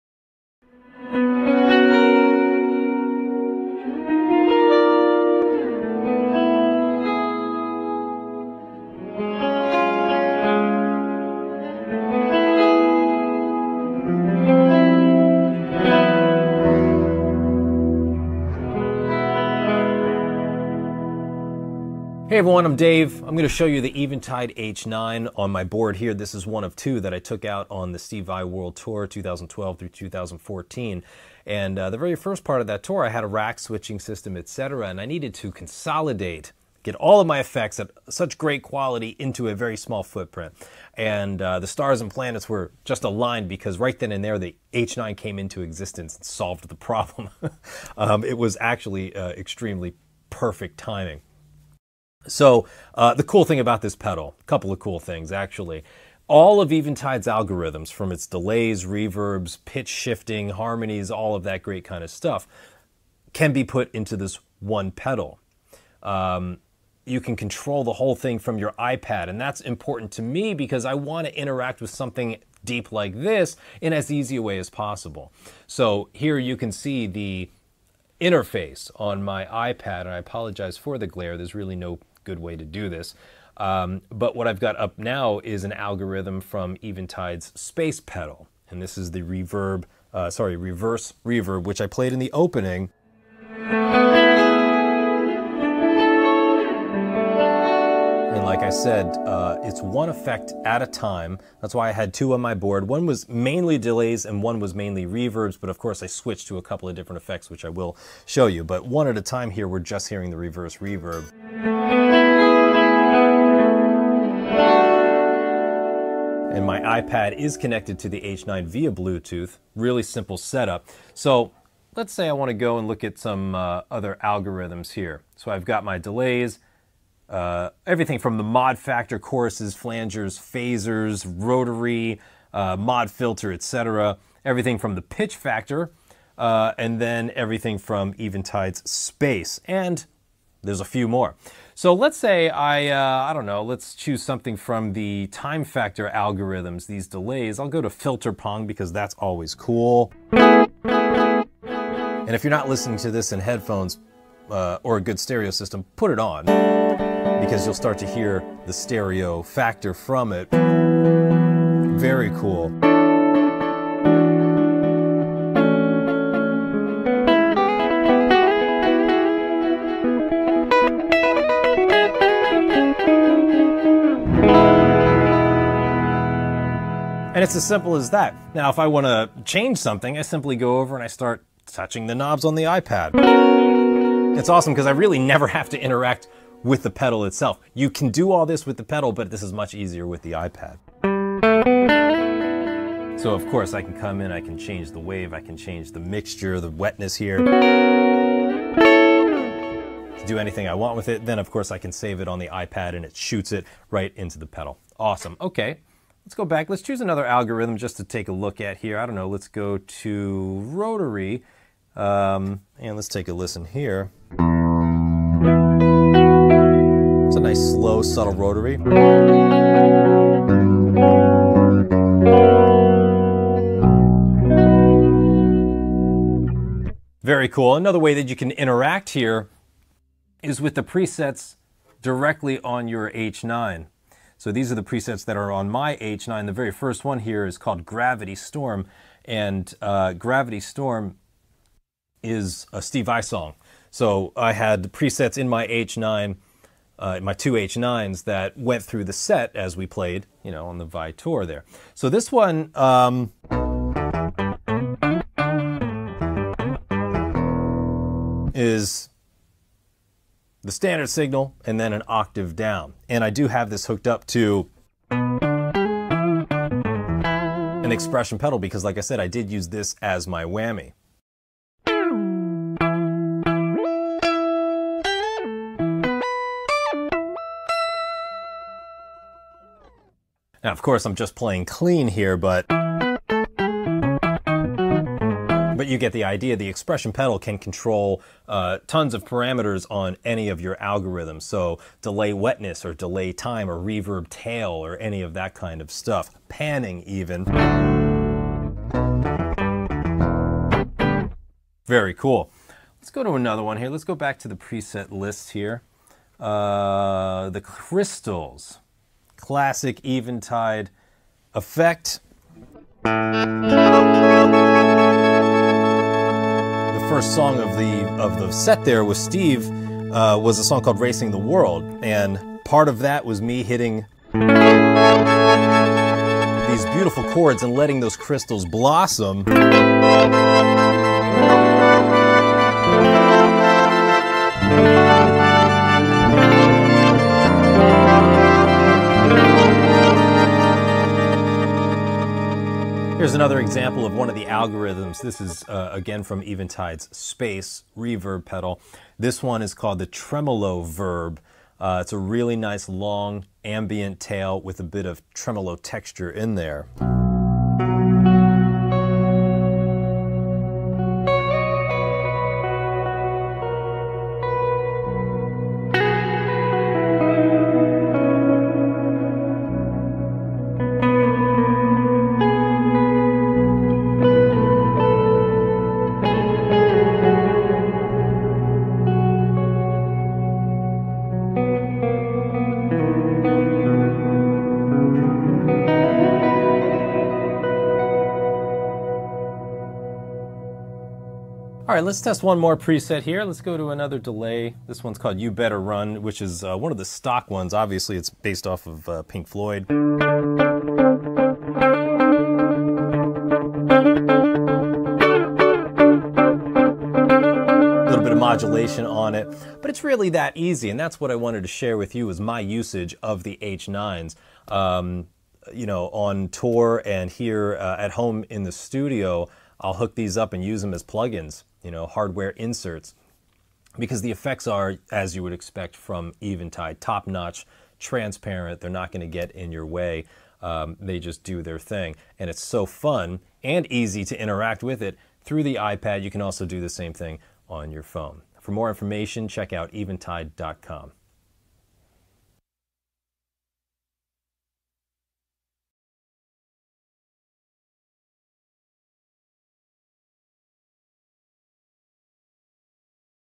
Dave Weiner讲解演示Eventide H9 Max
Dave Weiner讲解演示他如何使用Eventide H9 Max效果器。

微信公众号：Eventide效果器